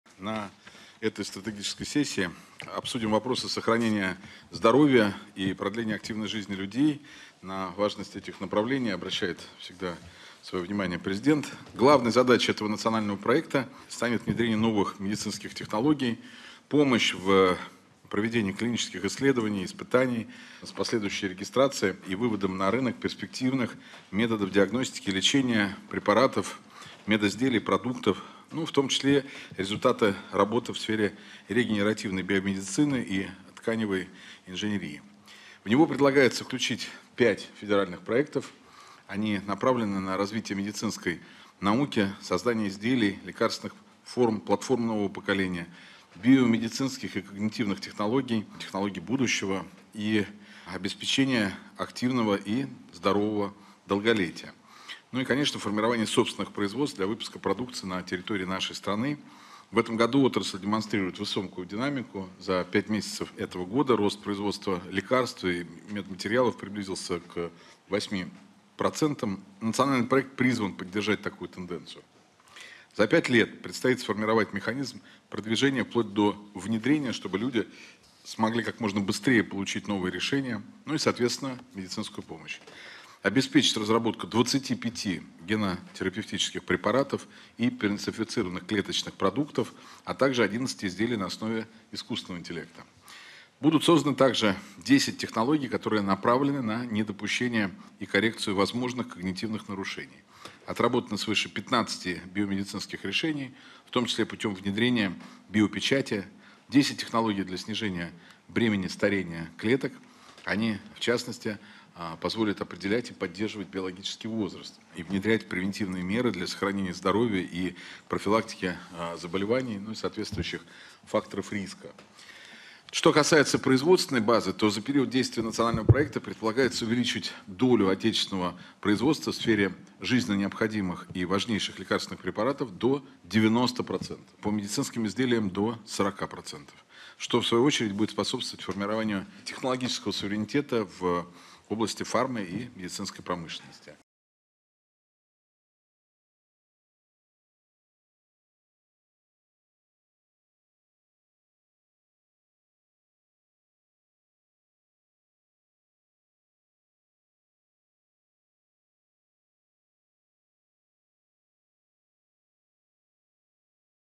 Стратсессия по нацпроекту «Новые технологии сбережения здоровья»
М.Мишустин: «Главной задачей этого национального проекта станет внедрение новых медицинских технологий, помощь в проведении клинических исследований и испытаний с последующей регистрацией и выводом на рынок перспективных методов диагностики и лечения, препаратов, медизделий и продуктов. В том числе результаты работы в сфере регенеративной биомедицины и тканевой инженерии».